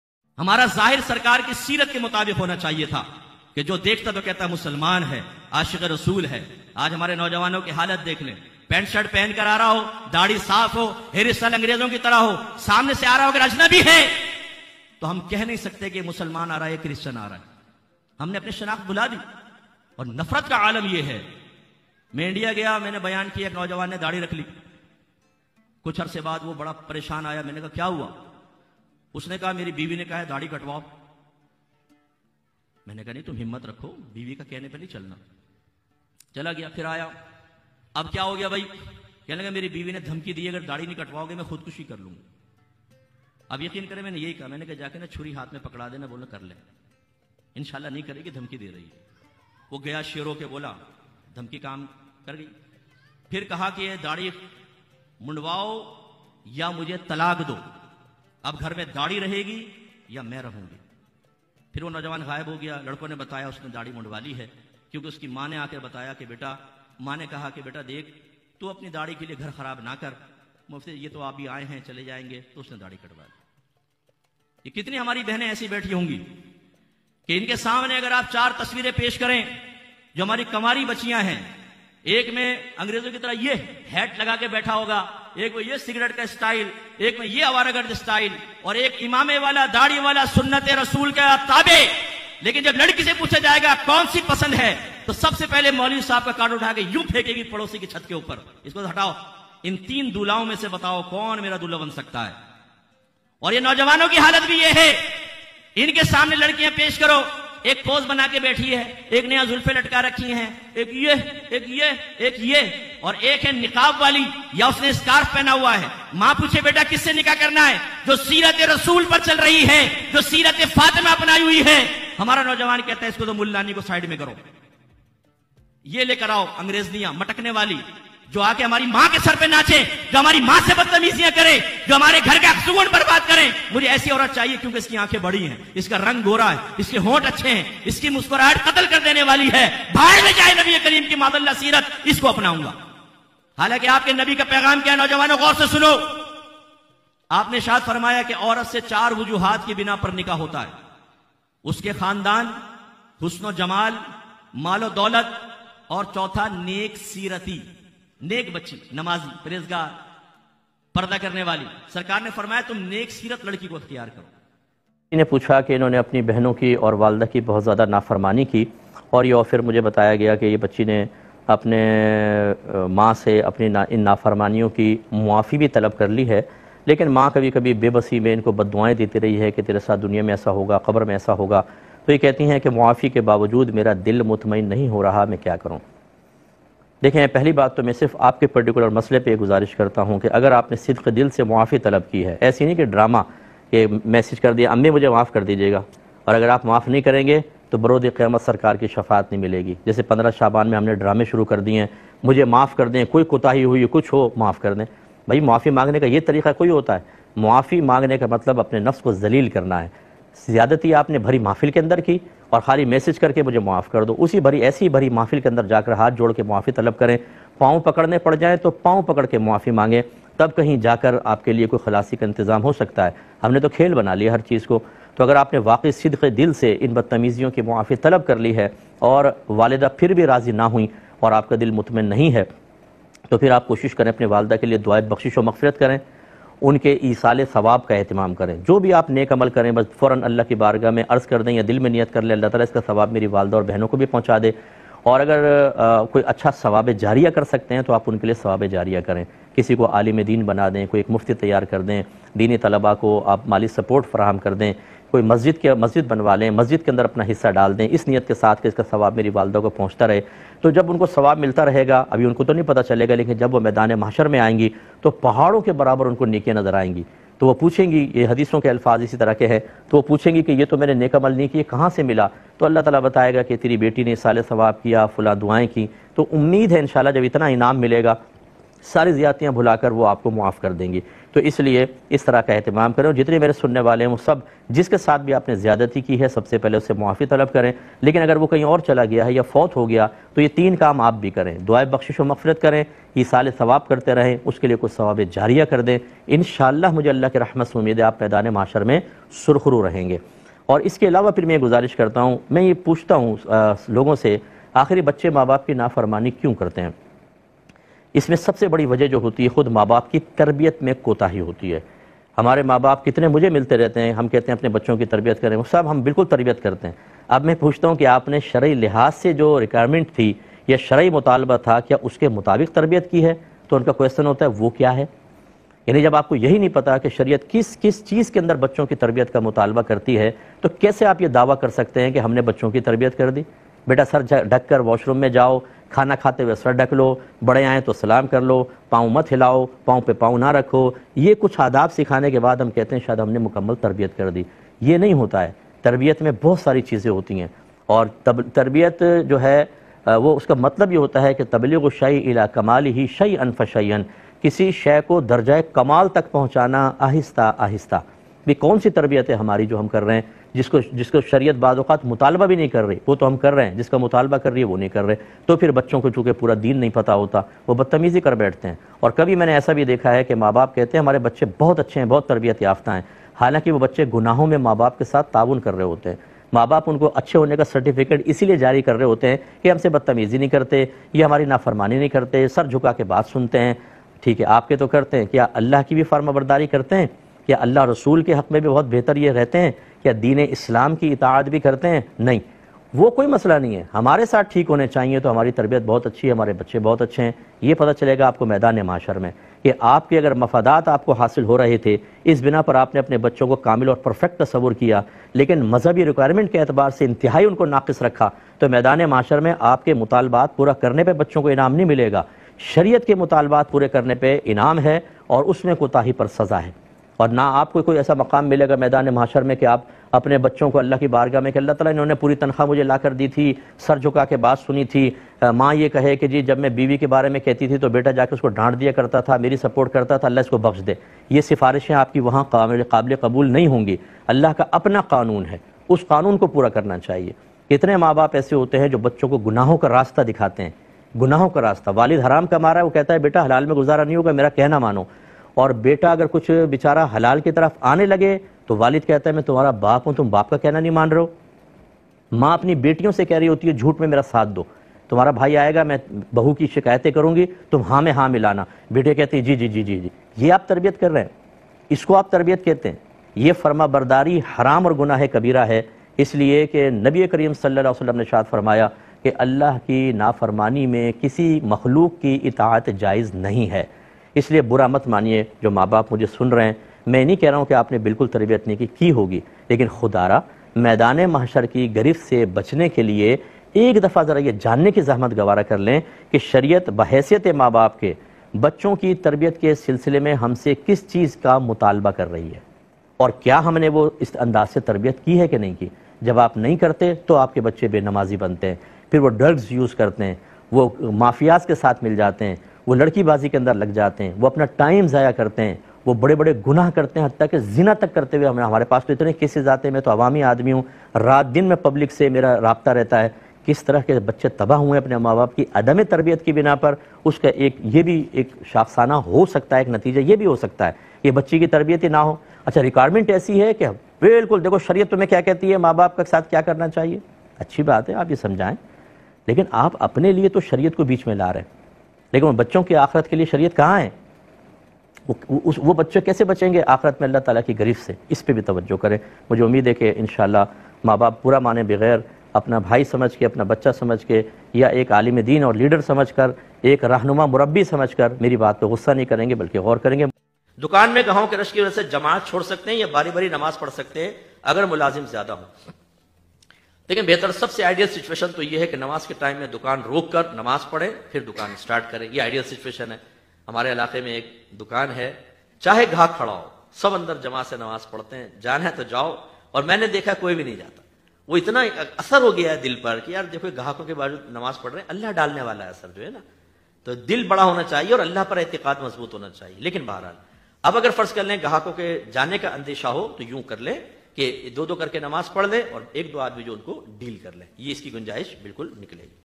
Aj Kal Hamary Naujawanon Ko Dekh Kr Pata Hi Nahin Chalta Keh Yeh Muslman Hain Ya Ghair Muslim
Chapters
1♥️0:00 Introduction🇵🇰♥️🇵🇸
2♥️2:45 Maa se bad'tameezi ke baad muaafi maang li magar maa phir bhi bad'duaaen deti hain ???
3♥️9:10 Dukan Par Rush Ki Waja Say Kya Jamat Tark Kr Sakte Hain?

    1♥️ آج کل ہمارے نوجوانوں کا حلیہ ایسا ہے کہ دور سے دیکھنے سے پتہ نہیں چلتا کہ مسلمان ہے یا غیر مسلم

2♥️ ماں سے بدم بد تمیزی کے بعد معافی مانگ لی مگر ماں پھر بھی بد دعائیں دیتی ہیں ؟

3♥️ دوکان پر گاہکوں کے رش کی وجہ سے کیا جماعت ترک کر سکتے ہیں؟